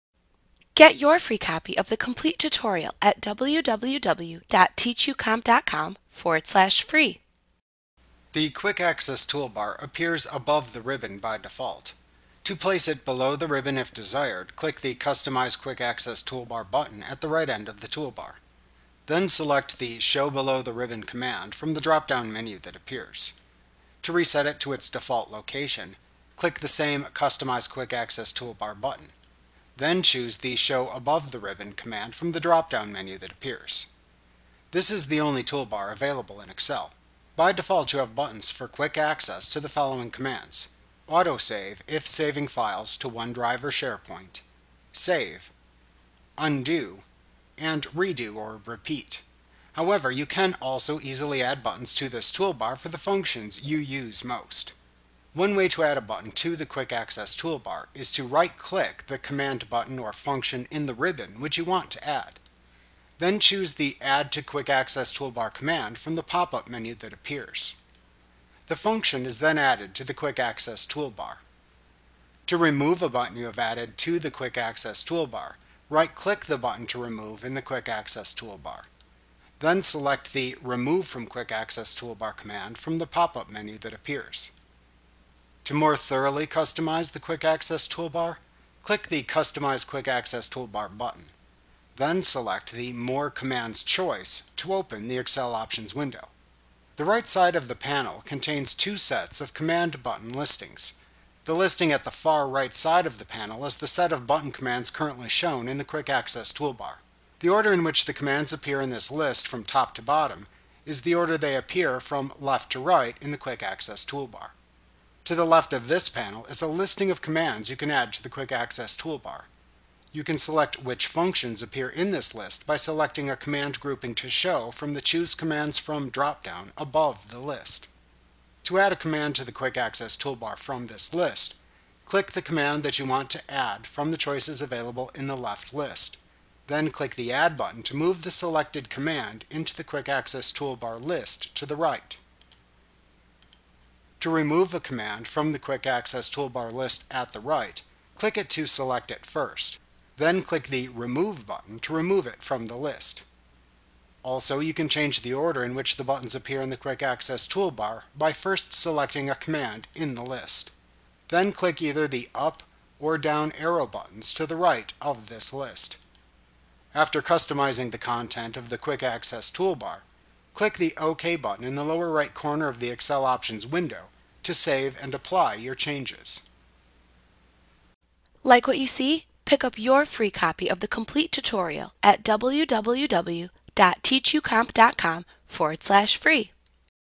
Excel 2019 & 365 Tutorial The Quick Access Toolbar Microsoft Training
FREE Course! Learn how to use the quick access toolbar in Microsoft Excel A clip from Mastering Excel Made Easy v. 2019 & 365. - the most comprehensive Excel tutorial available. Visit us today!